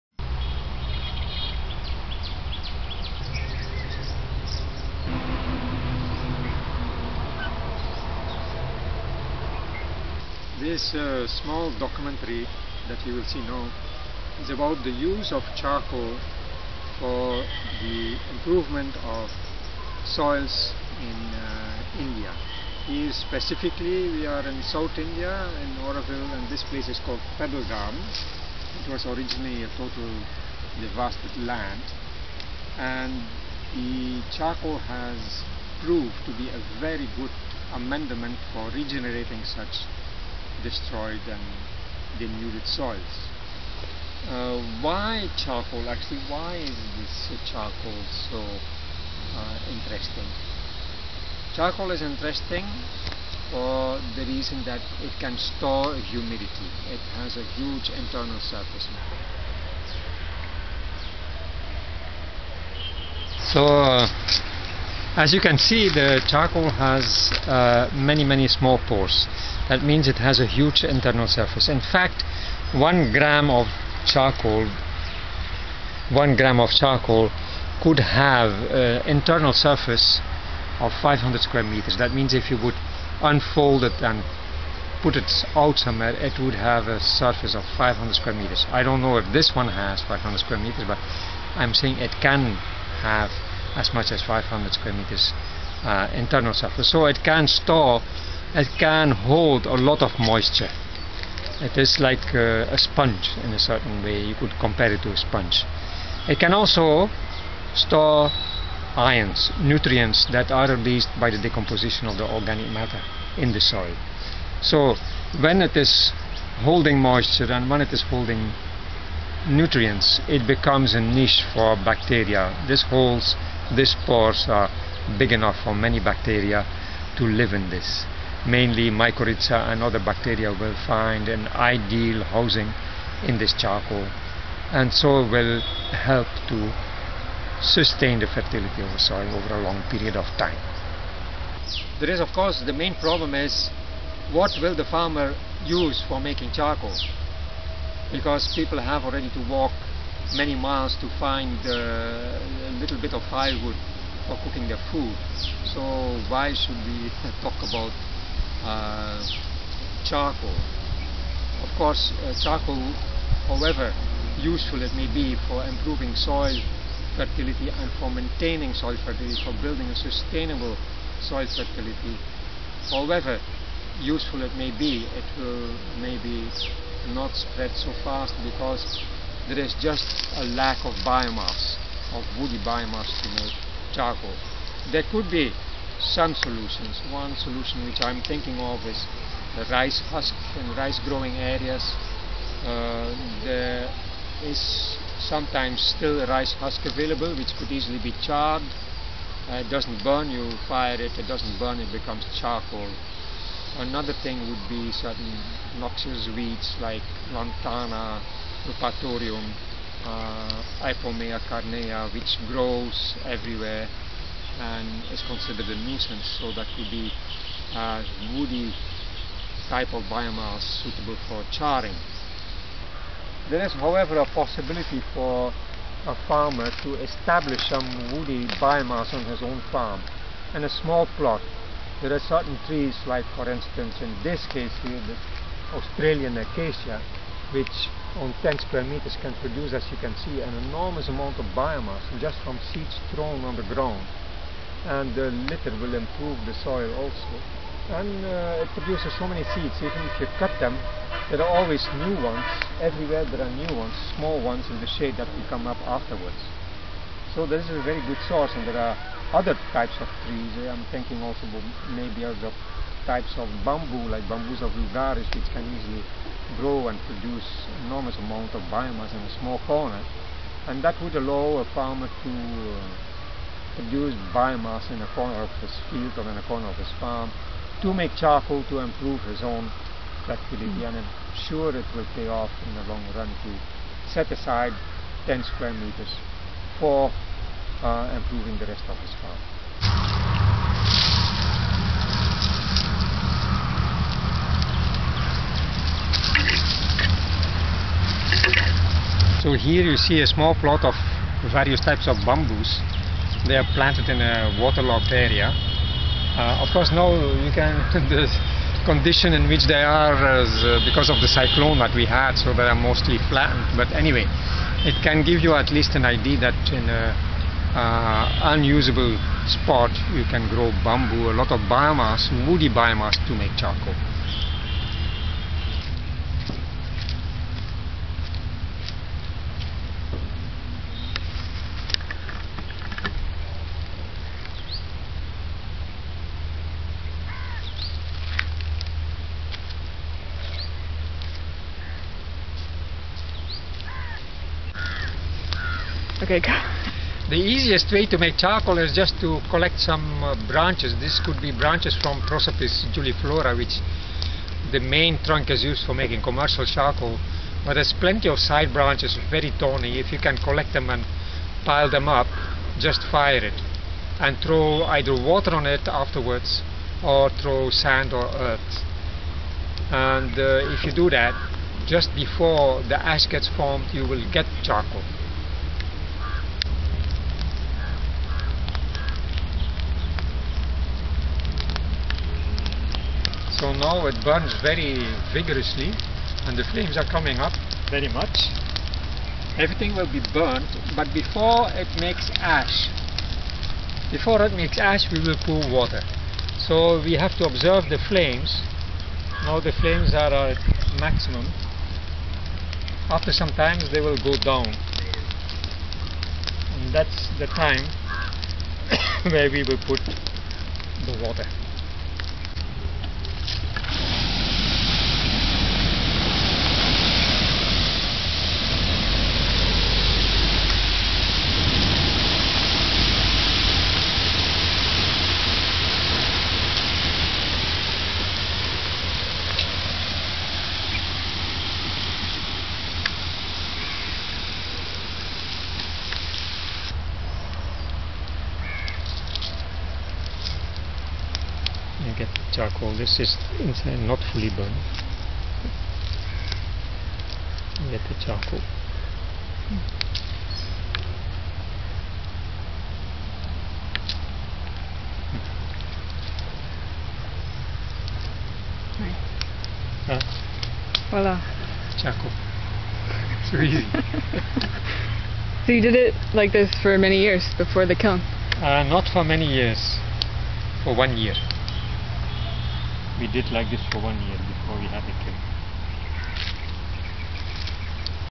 Charcoal at Pebble Garden - Part 1/3 - Charcoal for Depleted Soils
Bernard Declercq explains the role of charcoal in restoring soil fertility and demonstrates a simple method of making charcoal.